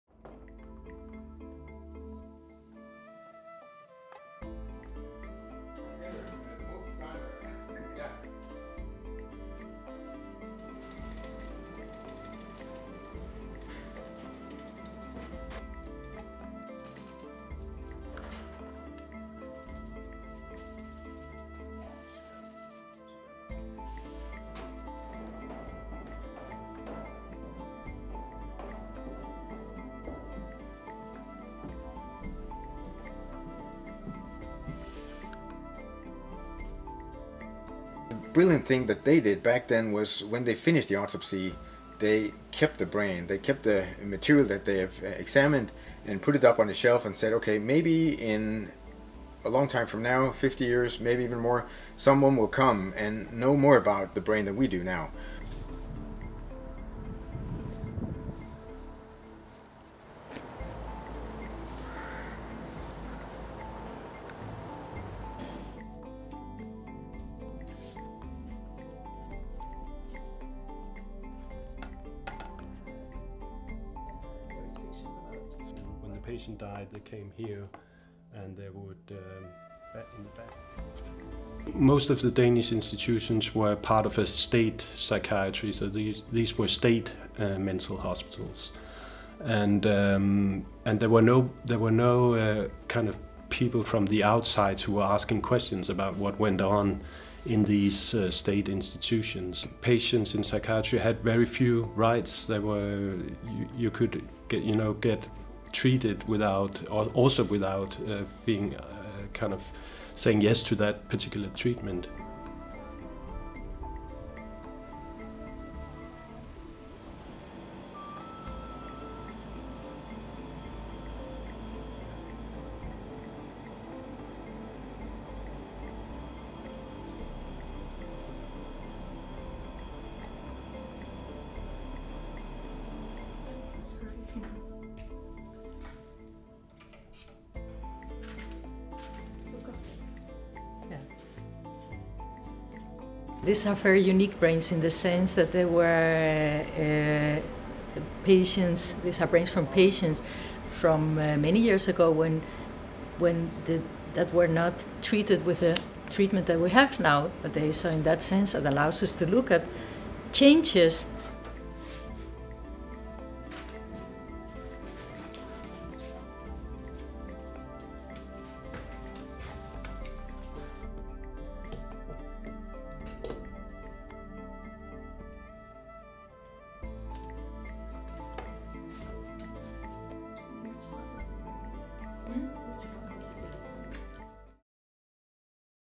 Au Danemark, près de 10 000 cerveaux humains conservés pour l’étude des maladies mentales
Dans le sous-sol de l'université danoise d'Odense se trouve une collection un peu particulière. Elle compte près de 10 000 cerveaux humains conservés dans du formol. Ces organes ont été prélevés lors d'autopsies de malades mentaux morts dans des hôpitaux psychiatriques du pays entre 1945 et 1982. Ils sont aujourd'hui utilisés pour la recherche médicale, bien qu'ils aient été prélevés à l'époque sans l'accord des patients ou de leur famille.